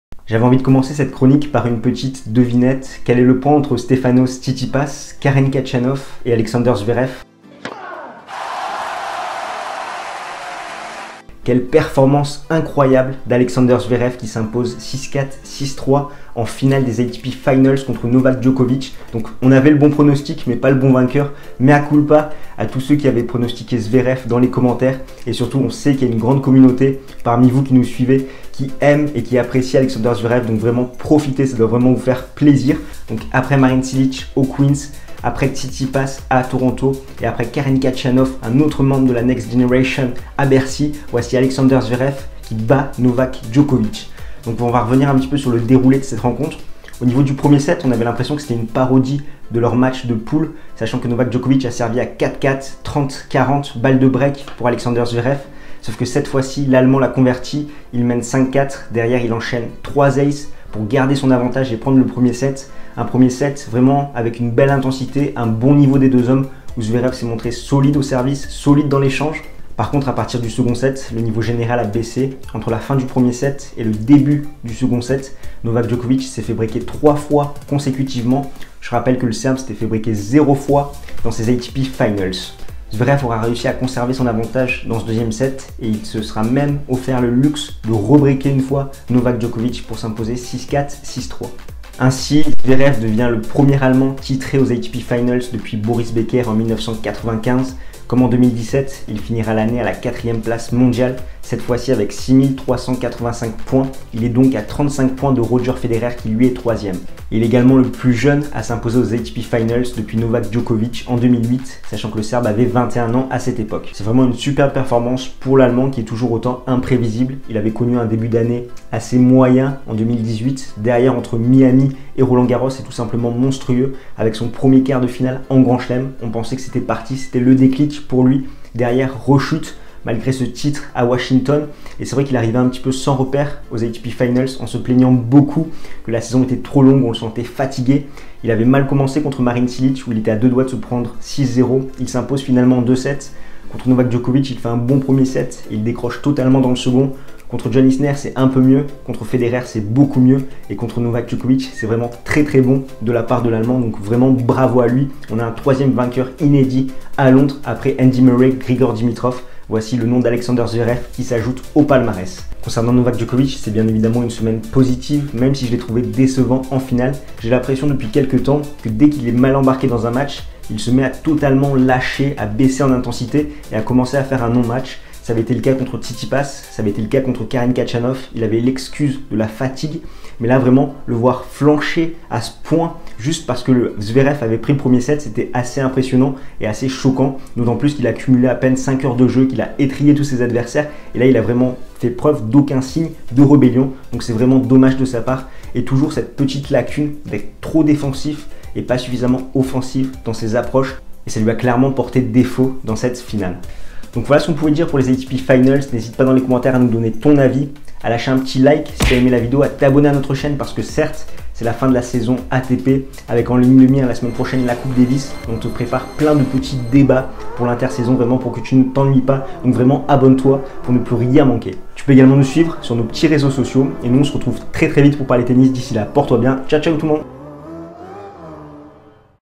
ALEXANDER ZVEREV S'OFFRE NOVAK DJOKOVIC | ATP Finals 2018
Alexander Zverev s'offre Novak Djokovic en finale des ATP Finals 2018 ! L'Allemand s'impose 6-4 6-3 dans cette ultime rencontre à Londres. Il n'aura cessé de surprendre en hissant son niveau de jeu au fil des matchs après des victoires contre Marin Cilic, John Isner et Roger Federer. Le Serbe aura eu une semaine positive où il se sera imposé contre John Isner, Marin Cilic et Kevin Anderson malgré tout cela n'aura pas suffi en finale !